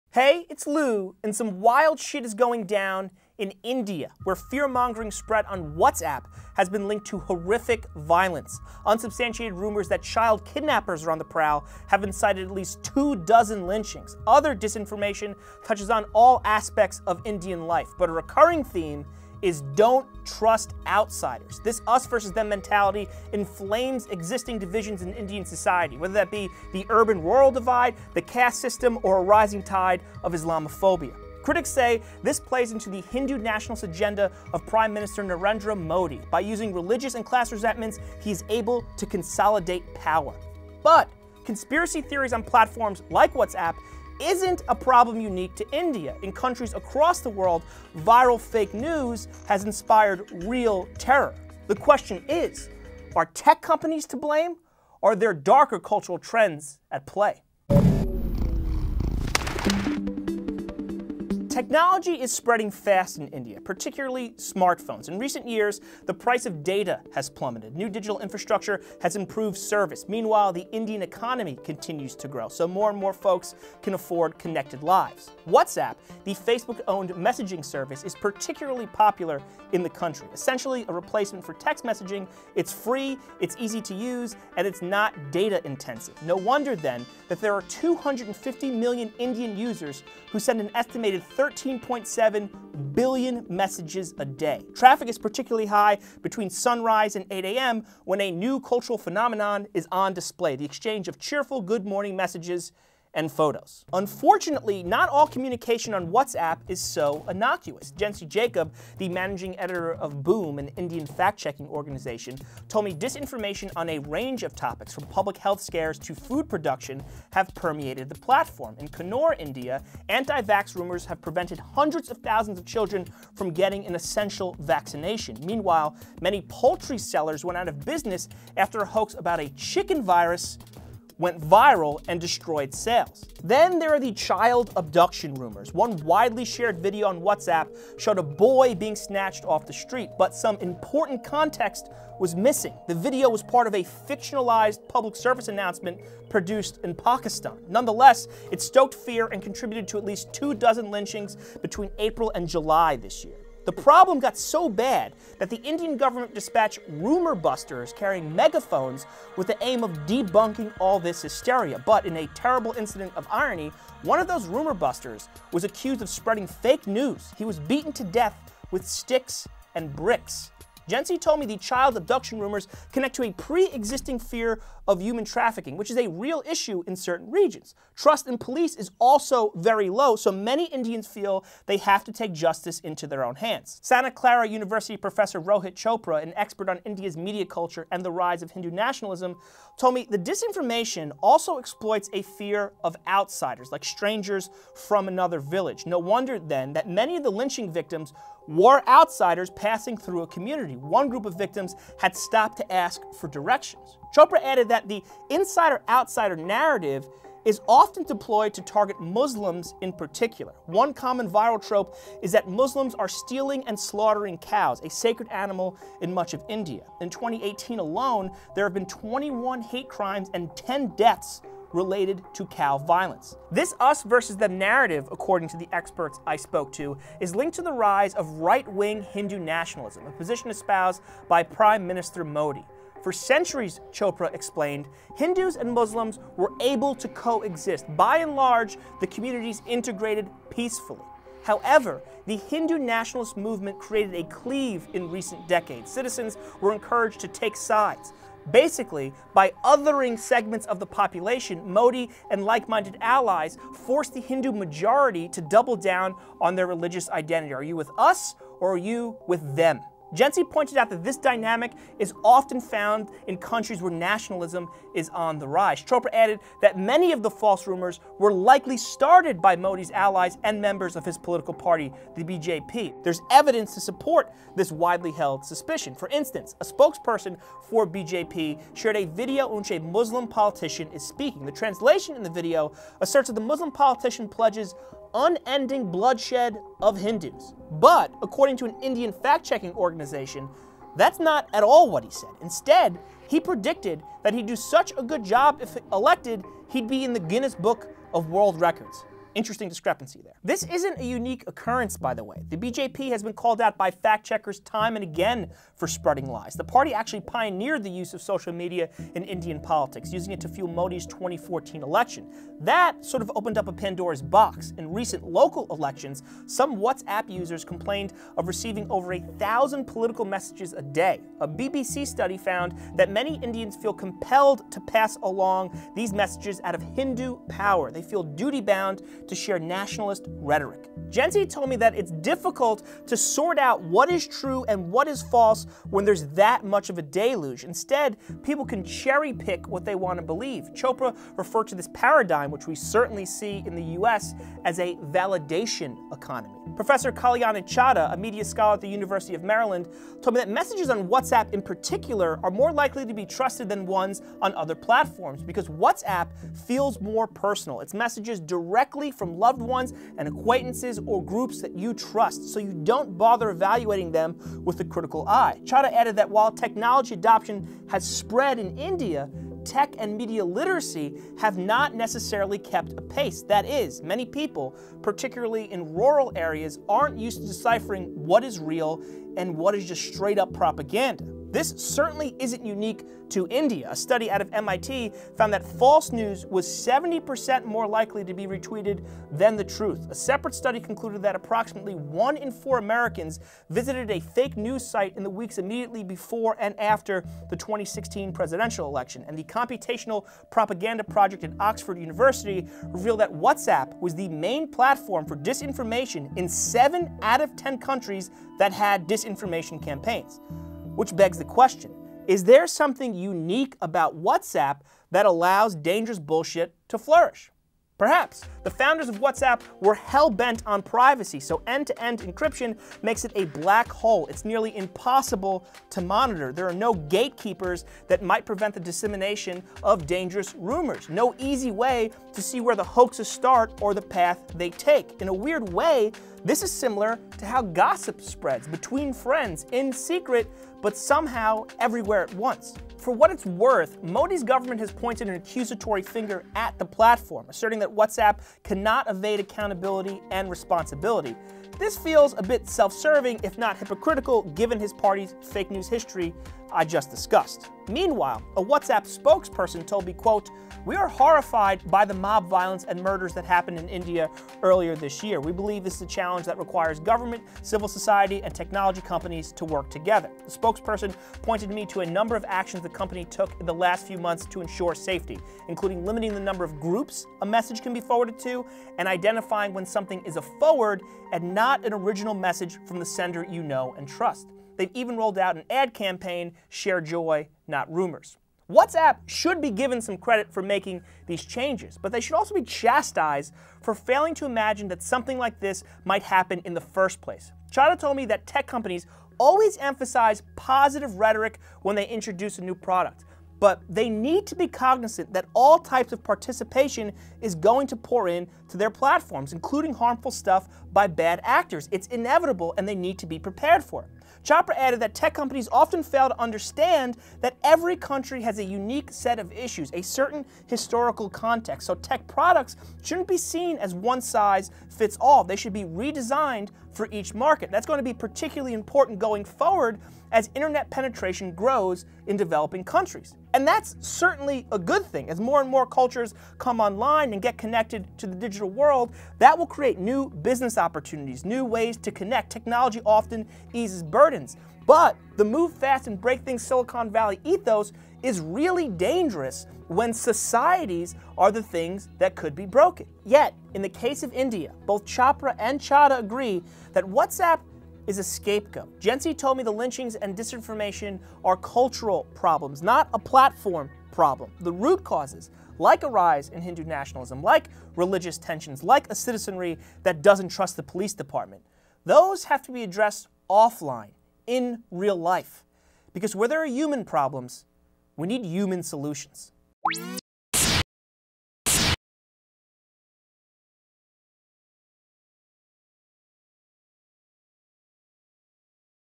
India’s WhatsApp lynchings
Disinformation is spreading like wildfire in India - viral rumors led to the lynchings of at least 25 people in 2018. Much of the fake news is being disseminated via WhatsApp. Lou examines the platform’s role in the mob violence, and explains why the rise of Hindu nationalism is fueling the violence.

BBC on Duty, Identity, Credibility: ‘Fake News’ and the ordinary citizen in India

CREDITS
Writer: Louis Foglia
Editor: m.cho
Researcher: Dushyant Naresh
Supervising Producer: Allison Brown